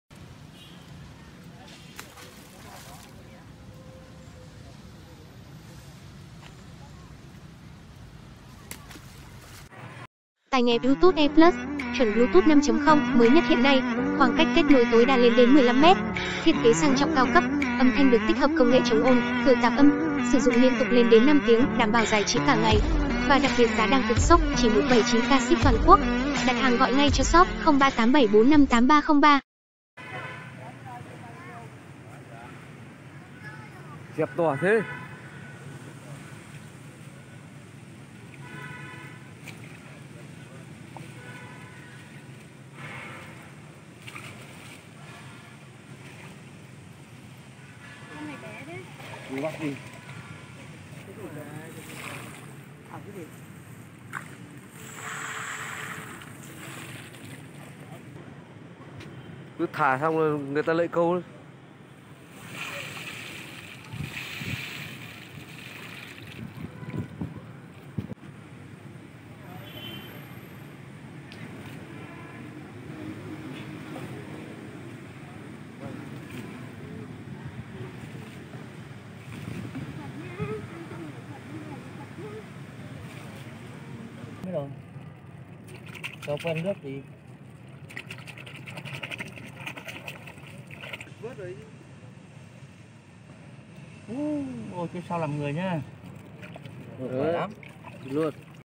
Phóng sinh 'đủ món' ra hồ Hoàn Kiếm ngày rằm tháng 7
Nhiều người dân mang cá chép, lươn, ốc... cả gạo mang ra hồ Hoàn Kiếm phóng sinh những ngày rằm tháng 7.Xem thêm